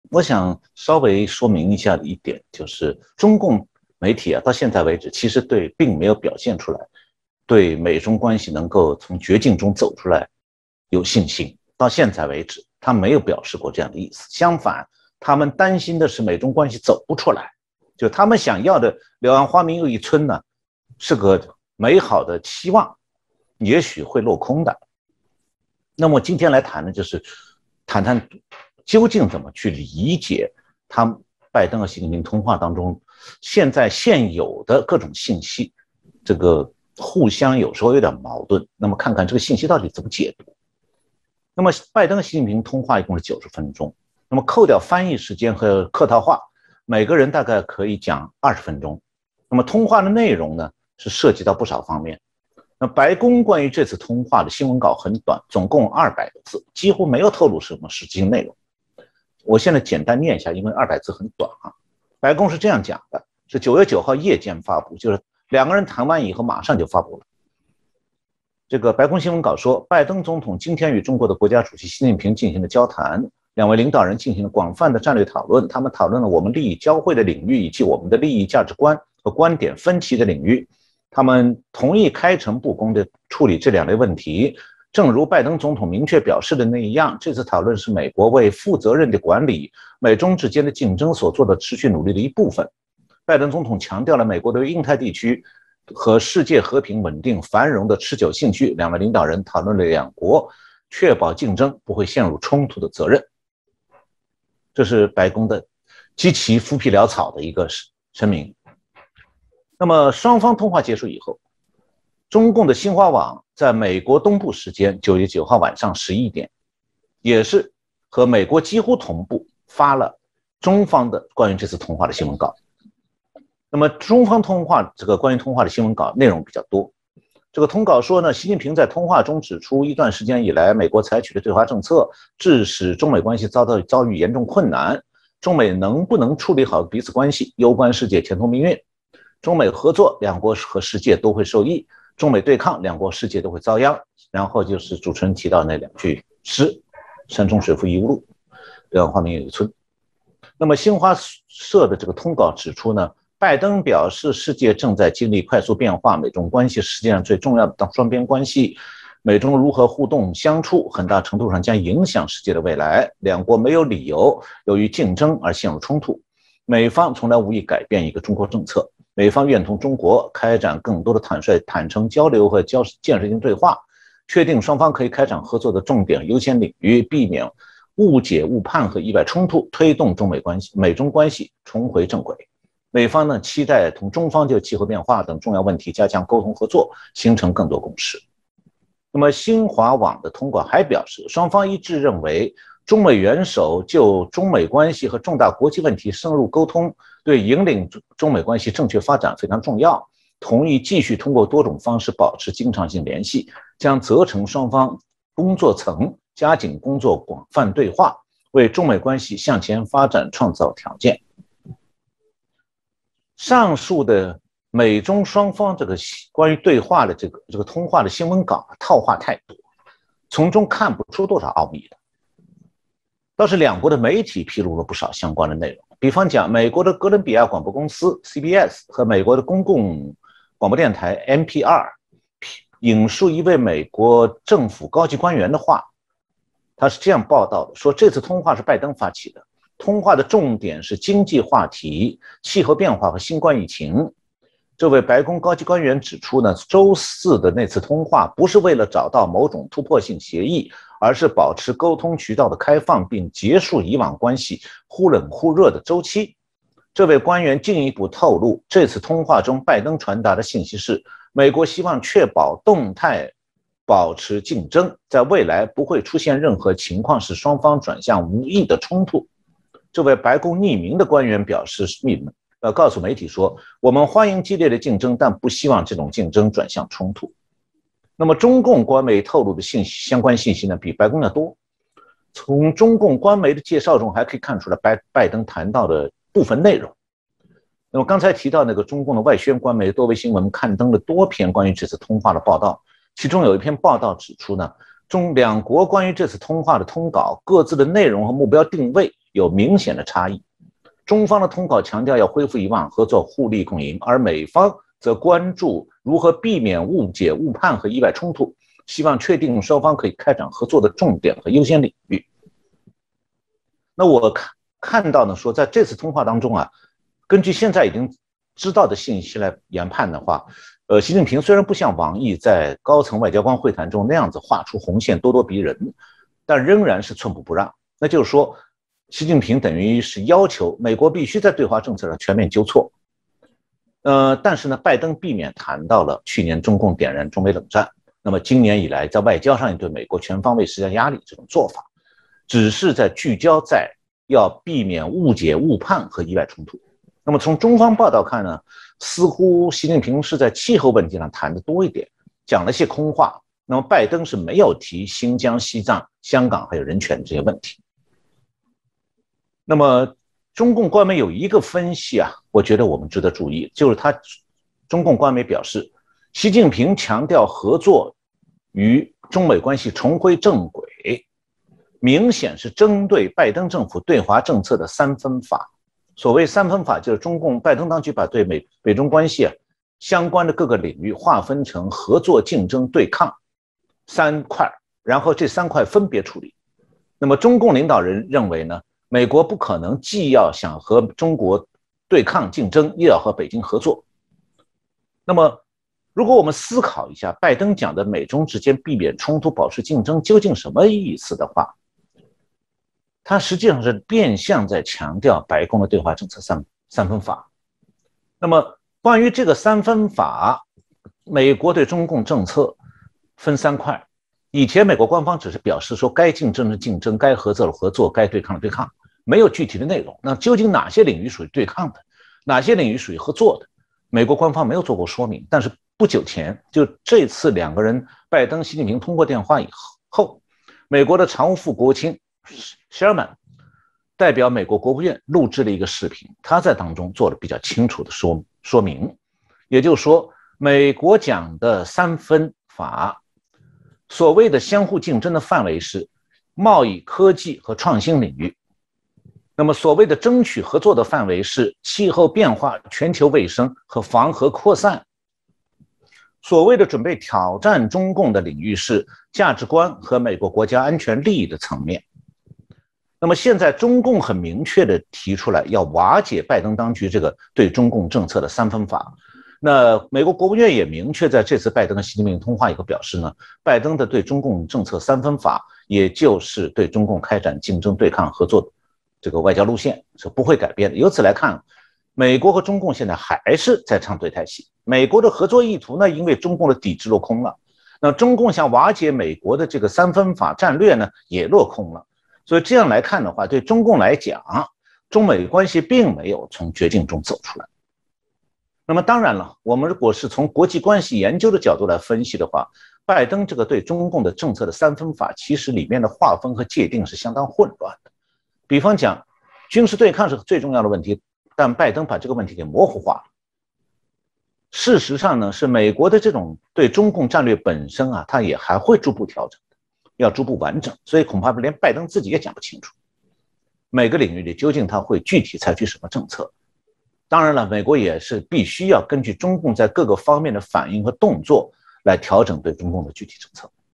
[ 程曉農0922精華 ] 習近平吟詩!曝美中現況。中國官媒大肆報導 美官員諷:作秀
主持人: 張宏林
國際時事議題:拜習通話 話中有話
拜登示好?示弱? 二度通話 話中有話 互相認知不同
兩任國安團隊觀點相通 中共利用美國 謀取代美戰略
習近平吟詩柳暗花明又一村?中媒大肆報導美方示好
拜登重申無意改變一中政策 台美升溫白開心?
中共喊”住房不炒” 地產巨頭恆大先破產 骨牌效應第一張?

本集來賓:
中國經濟學家 旅美學者  程曉農